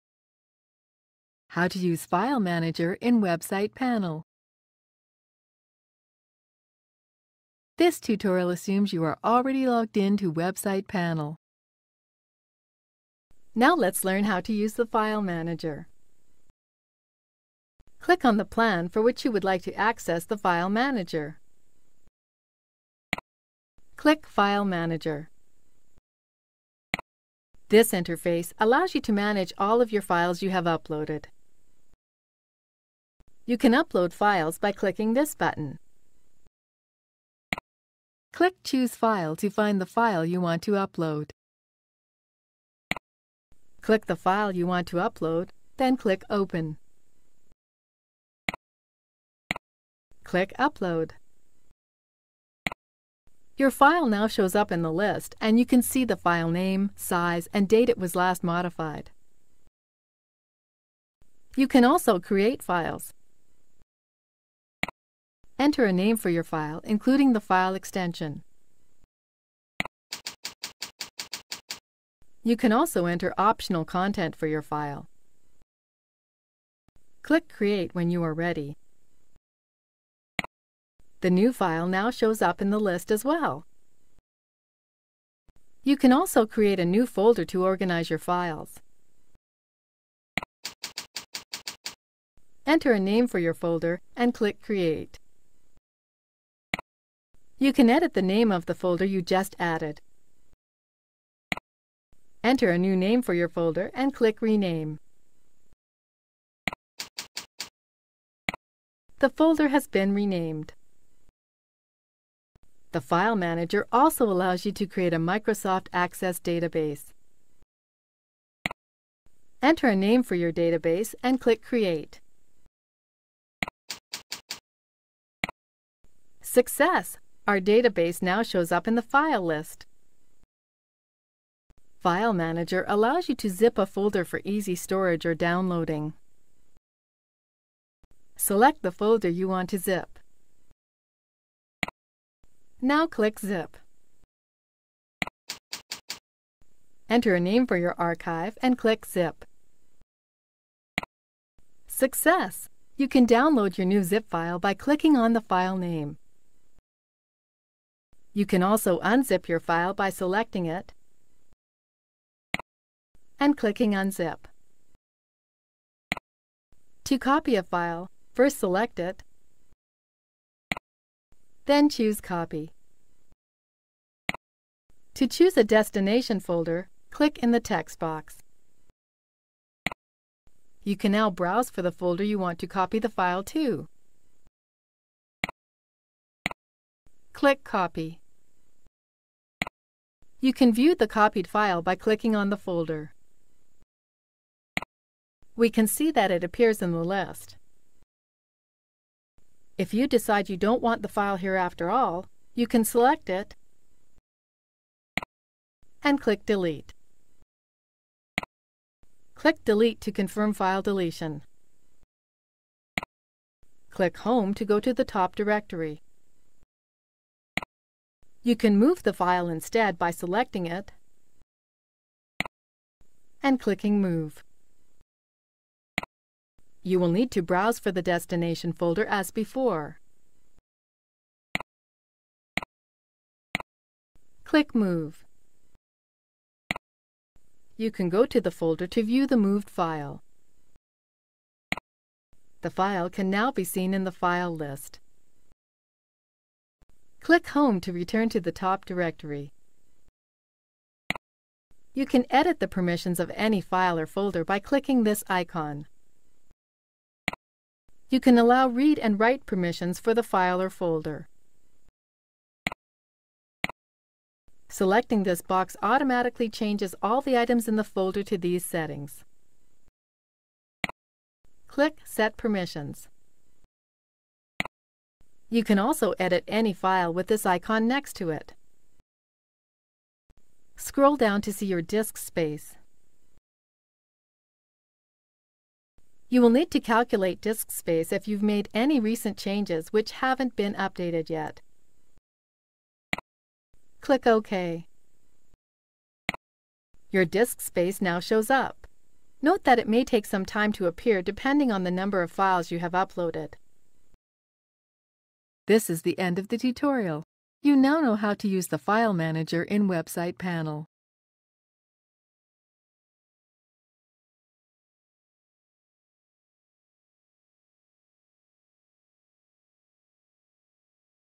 How to use File Manager in WebsitePanel
• Log into WebsitePanel.
• Click on the plan for which you want to access file manager.
• Go to file manager option on left window. The interface will allow you to manage all your uploaded files.
• Click on Upload tab to upload new files. Choose file(s) you want to upload from your hard drive and click on Upload button, when done. Your file will show up in the list and you can see file name, size and last modified date.
• Click on Create Files tab for creating new files. Enter file name with extension and content inside it. Click on Create button to continue. File will be created.
• Click on Create Folder to add a new folder for file organization. Fill folder name and click on create button. Folder will show up in the list. You can rename the folder by clicking on the icon next to name.
• Click on Create Access DB to create a Microsoft Access database. Enter database name and your database will show up in the list.
• To zip a folder, select it by checking the box with its name and click on Zip icon from top list. Similarly, you can unzip folders by checking their box and clicking on Unzip icon from top.
• Click on Copy tab to copy a folder at some other location. Select the file to copy and click on Copy icon from top. Browse the folder where you want to copy file. Click on Copy button. You can view the copied file by checking the folder.
• Click on Move tab to move a file to some other location. Remember, the file once moved will be seen only at destination and removed from actual path.
• To delete files/folders, select the file/folder you want to delete. Click on Delete button at top to continue.
• Go to Home to go to top of directory.
• To edit permissions of a file, click lock icon with any folder or file. Click on Set Permissions, when done.
• To edit any file, click on edit icon next to its name.
• You can know diskspace by scrolling down and clicking on Calculate Diskspace button.